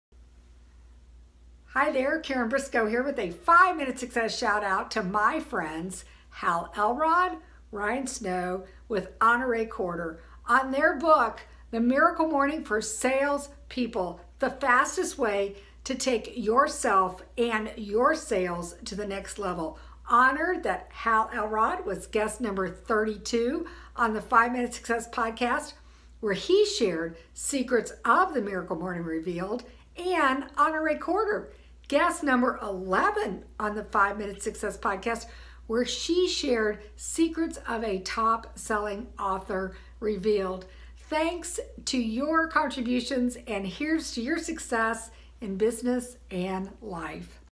Hal Elrod - Ryan Snow - Honoree Corder - Miracle Morning for Sales People
I was honored to have both Hal Elrod, episode 32, AND Honoree Corder, episode 11, as guests on the 5 Minute Success Podcast. Their collaboration on this book is absolutely invaluable!